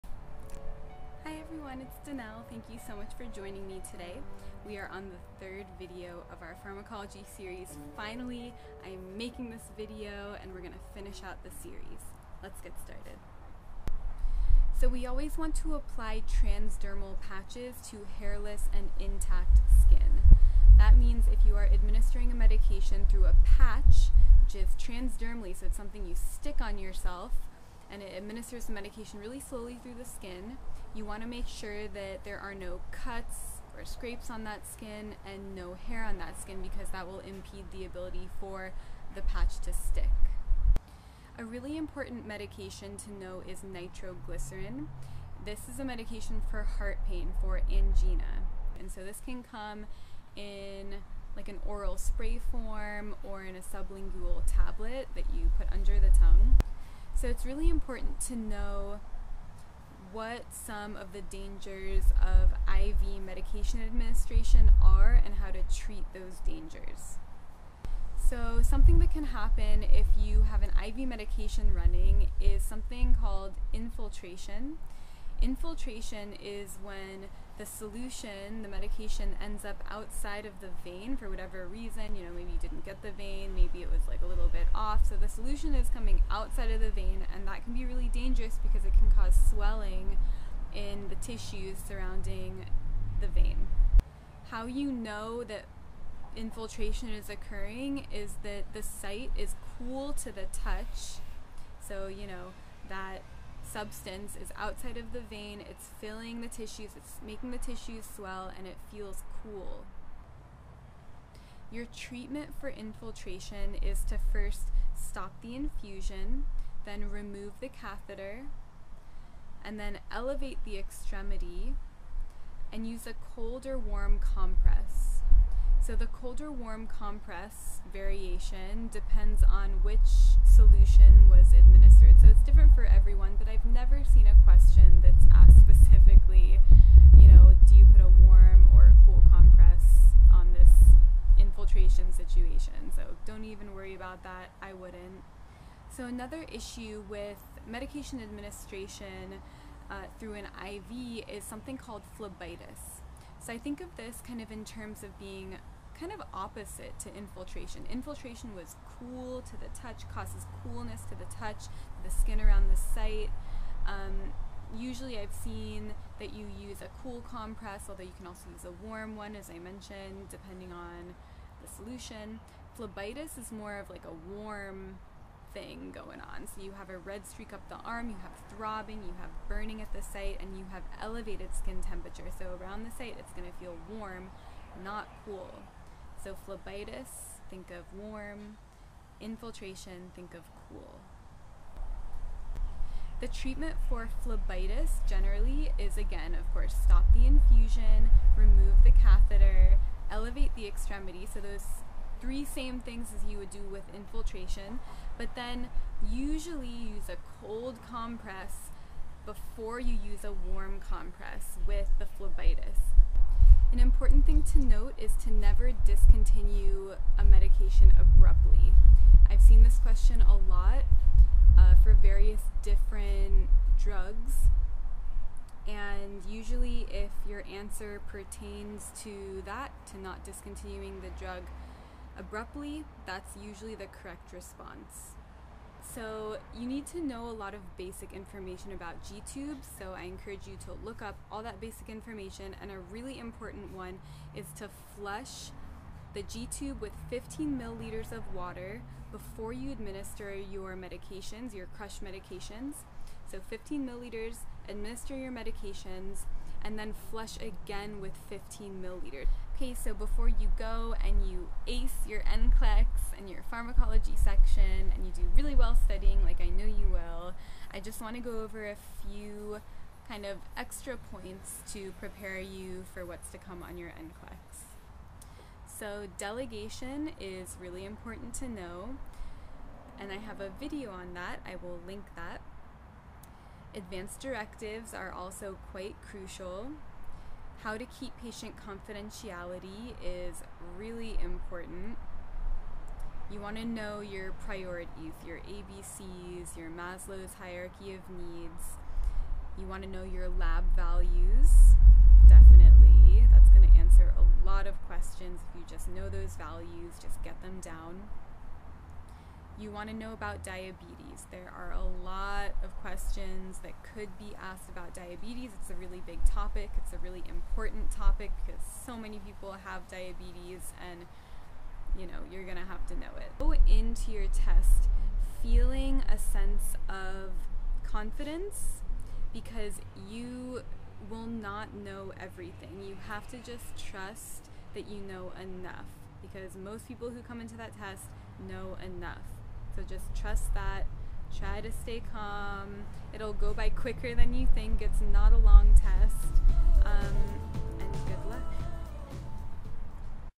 *5 Minute Pharmacology: Part 3* *Studied Pharmacology Only 1.5 Days and Passed NCLEX*
Here is your FINAL VIDEO in our 3-part series: 5 Minute Pharmacology. YAY!!!

These videos are intended to give a brief and easy overview of how to study for pharmacology to pass the NCLEX and what you definitely need to know.

0:13 Transdermal Patches
0:41 Nitroglycerin
0:58 IV dangers
1:14 Infiltration
1:53 Treatment for Infiltration
2:33 Phlebitis
3:13 Treatment for Phlebitis
3:35 Never discontinue meds abruptly
3:57 G-tubes
4:35 EXTRA TIPS for the rest of the NCLEX exam